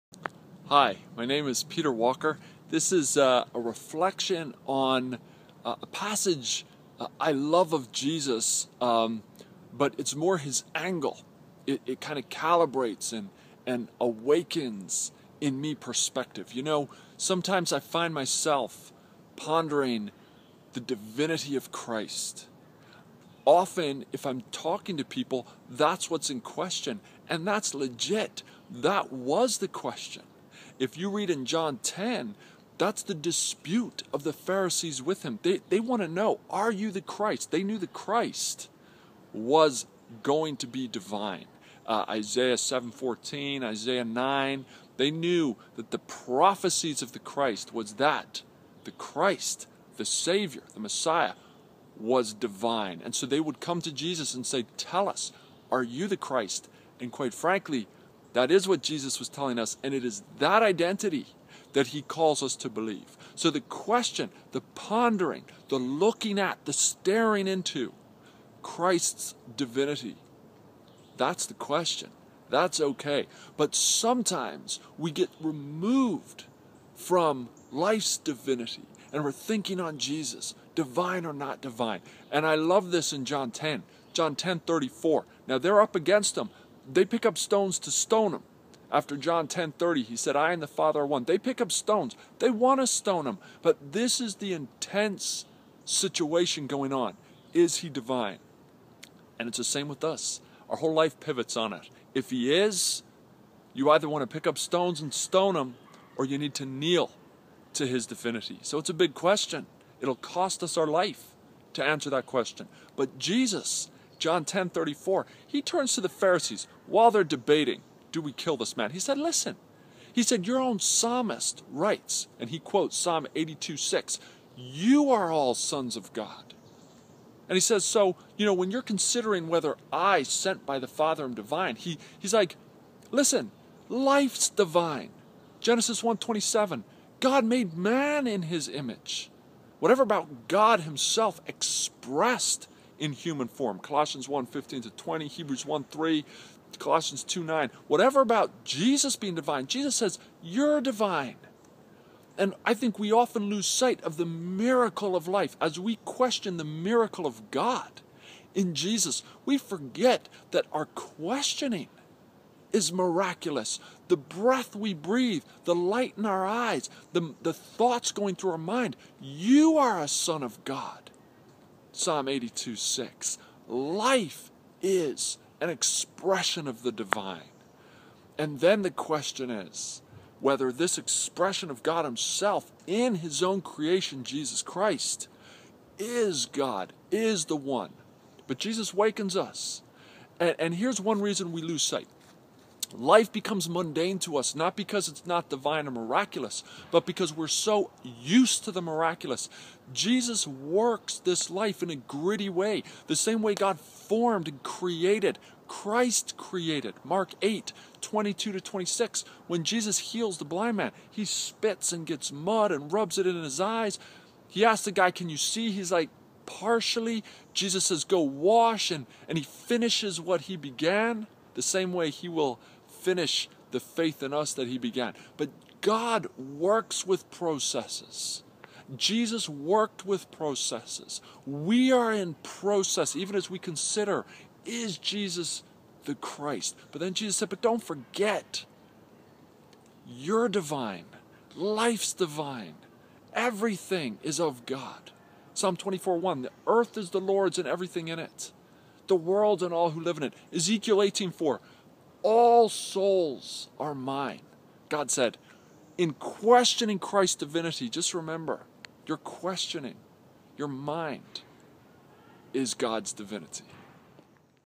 YOU ARE gODS, by Peter Walker
When considering Christ's divinity, He reminds us to remember our own...

John 10:34
Psalm 82:6
Mark 8:22-26
Genesis 1:27